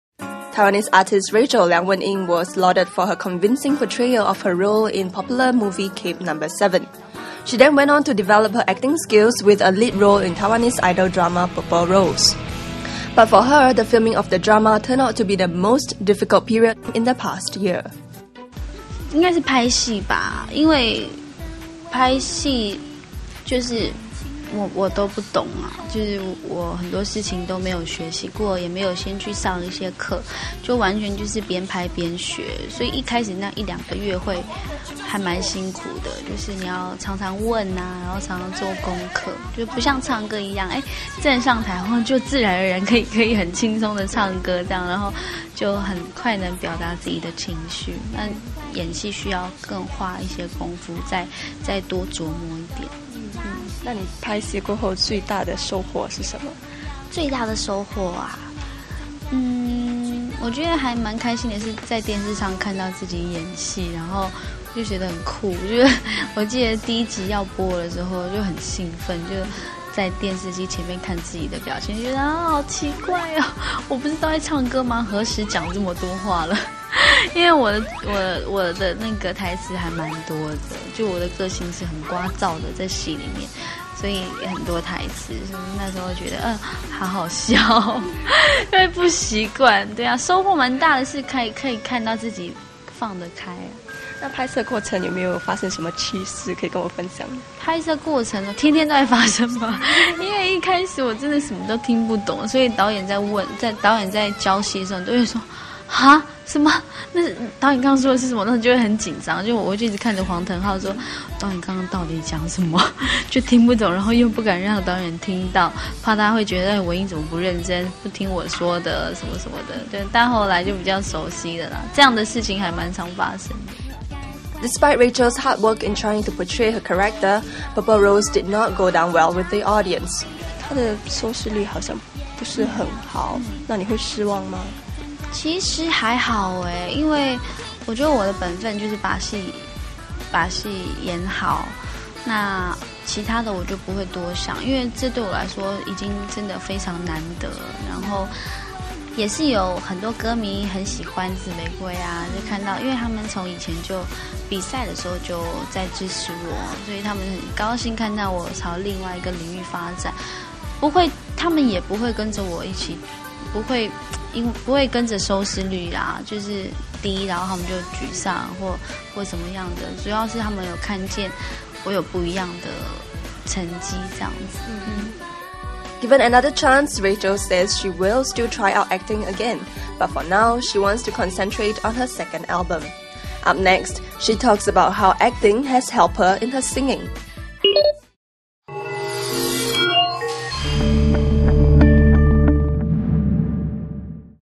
Rachel Liang Pt 4: Debut drama a flop
Singer Rachel Liang Wen Yin landed the lead role in idol TV drama "Purple Rose", despite not having any experience.Unfortunately, the show did not go down well with the audience...